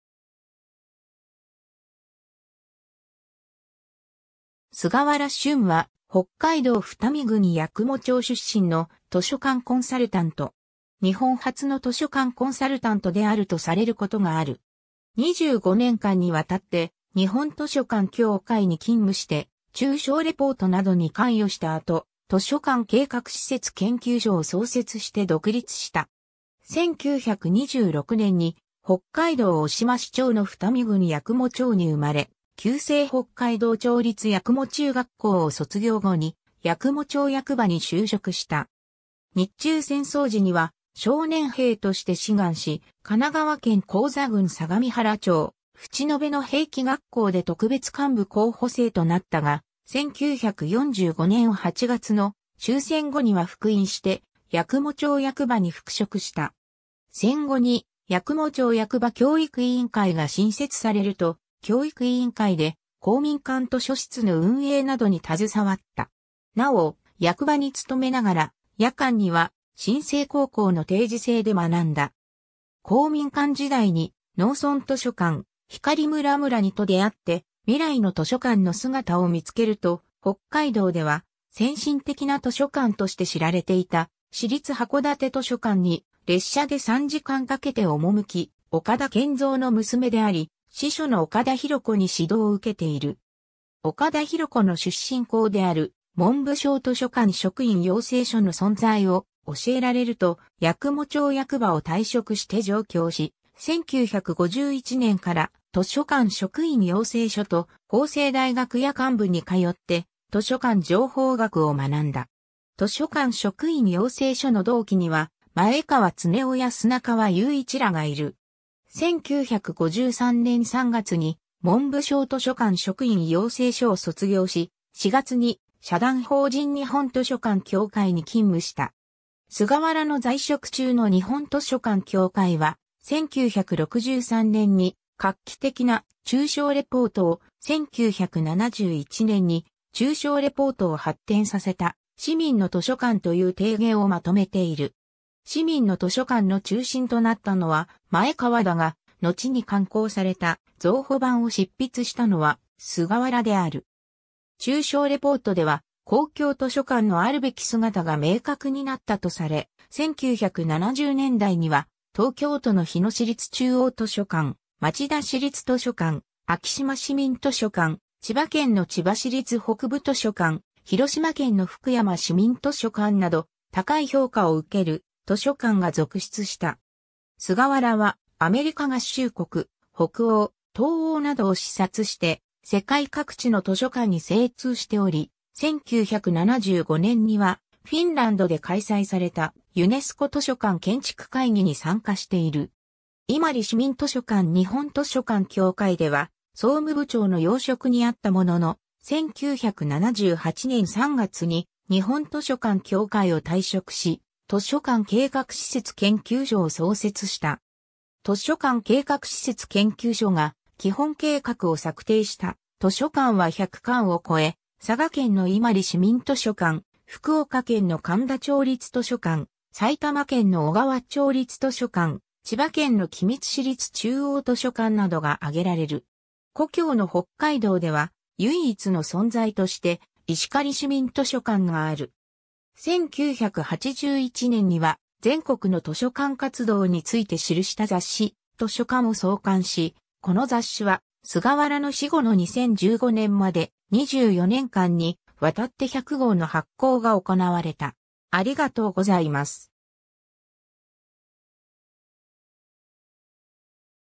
菅原峻
菅原 峻（すがわら たかし、1926年 - 2011年6月24日）は、北海道二海郡八雲町出身の図書館コンサルタント。
「日本初の図書館コンサルタント」であるとされることがある。
25年間にわたって日本図書館協会に勤務して『中小レポート』などに関与した後、図書館計画施設研究所を創設して独立した。
 1926年（昭和元年）に北海道渡島支庁の二海郡八雲町に生まれ、旧制北海道庁立八雲中学校を卒業後に八雲町役場に就職した。
日中戦争時には少年兵として志願し、神奈川県高座郡相模原町淵野辺の兵器学校で特別幹部候補生となったが、1945年（昭和20年）8月の終戦後には復員して八雲町役場に復職した。
戦後に八雲町役場教育委員会が新設されると、教育委員会で公民館図書室の運営などに携わった。
なお、役場に勤めながら夜間には新制高校の定時制で学んだ。
 公民館時代に『農村図書館』『光村々に』（柏葉書院）と出会って“未来の図書館の姿”を見つけると、北海道では先進的な図書館として知られていた市立函館図書館に列車で3時間かけて赴き、岡田健蔵の娘であり司書の岡田弘子に指導を受けている。
岡田弘子の出身校である文部省図書館職員養成所の存在を教えられると、八雲町役場を退職して上京し、1951年（昭和26年）から図書館職員養成所と法政大学夜間部に通って図書館情報学を学んだ。
図書館職員養成所の同期には前川恒雄（日野市立図書館初代館長、図書館情報学者）や砂川雄一（日野市立図書館第2代館長）らがいる。
 1953年（昭和28年）3月に文部省図書館職員養成所を卒業し、4月に社団法人日本図書館協会に勤務した。
菅原の在職中の日本図書館協会は、1963年（昭和38年）に画期的な『中小レポート』を、1971年（昭和46年）に中小レポートを発展させた『市民の図書館』という提言をまとめている。
『市民の図書館』の中心となったのは前川だが、のちに刊行された増補版を執筆したのは菅原である。
中小レポートでは公共図書館のあるべき姿が明確になったとされ、1970年代には東京都の日野市立中央図書館、町田市立図書館、昭島市民図書館、千葉県の千葉市立北部図書館、広島県の福山市民図書館など、高い評価を受ける図書館が続出した。
菅原はアメリカ合衆国、北欧、東欧などを視察して世界各地の図書館に精通しており、1975年（昭和50年）にはフィンランドで開催されたユネスコ図書館建築会議に参加している。
 伊万里市民図書館 日本図書館協会では総務部長の要職にあったものの、1978年（昭和53年）3月に日本図書館協会を退職し、図書館計画施設研究所を創設した。
図書館計画施設研究所が基本計画を策定した図書館は100館を超え、佐賀県の伊万里市民図書館、福岡県の苅田町立図書館、埼玉県の小川町立図書館、千葉県の君津市立中央図書館などが挙げられる。
故郷の北海道では唯一の存在として石狩市民図書館がある。
1981年（昭和56年）には全国の図書館活動について記した雑誌『としょかん』を創刊し、この雑誌は菅原の死後の2015年（平成27年）まで、24年間にわたって100号の発行が行われた。